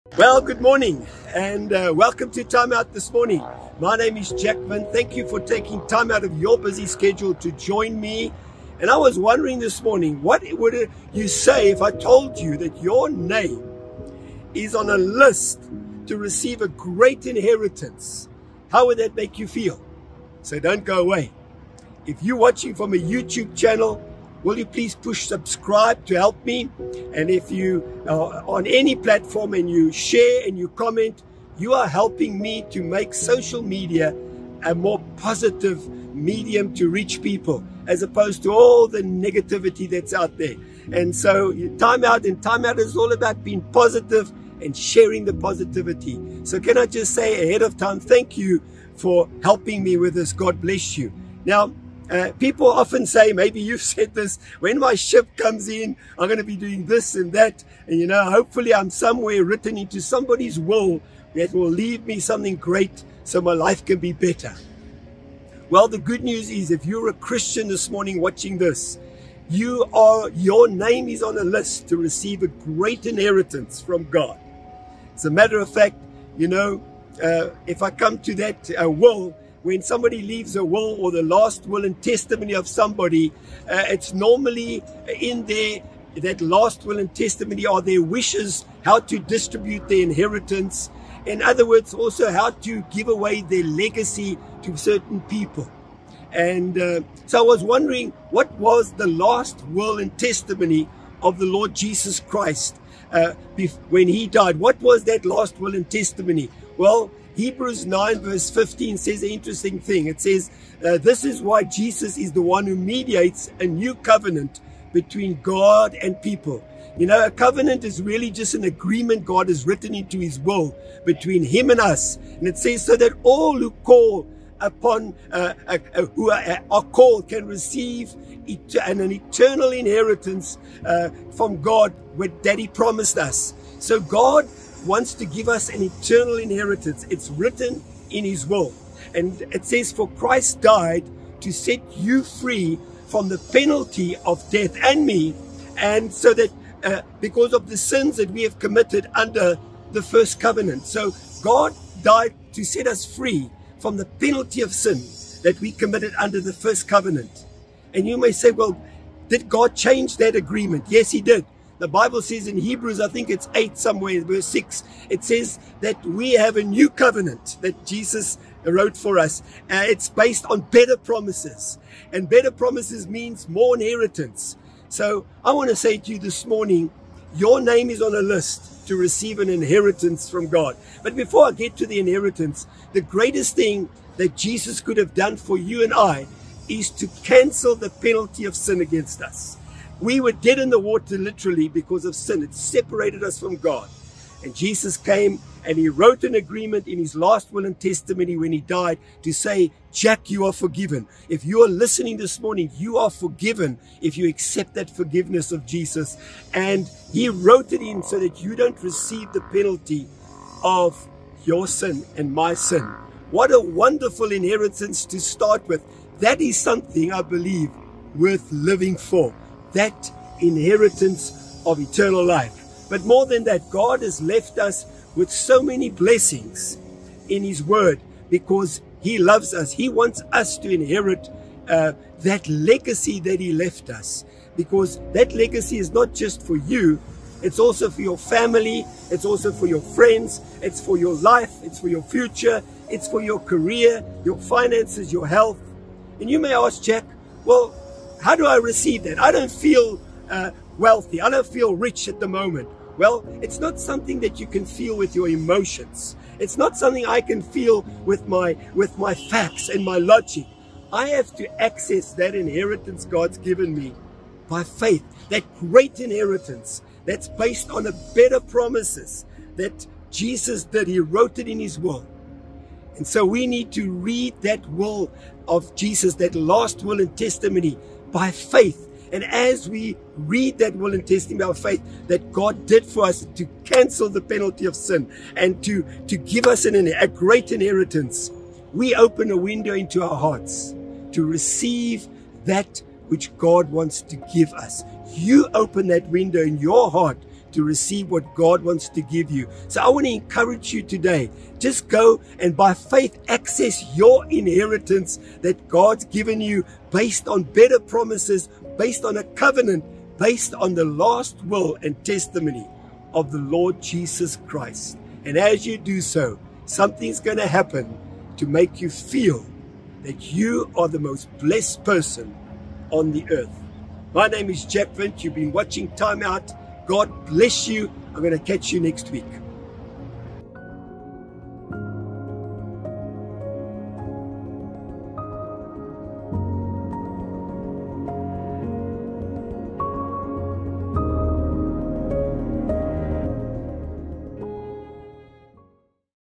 Jesus left me in His will | Jack Windt | Timeout
Jesus left me in His will

Watch now and find out how to claim your inheritance from God.
Grab a cup of coffee and enjoy some timeout.☕🕖

EP.155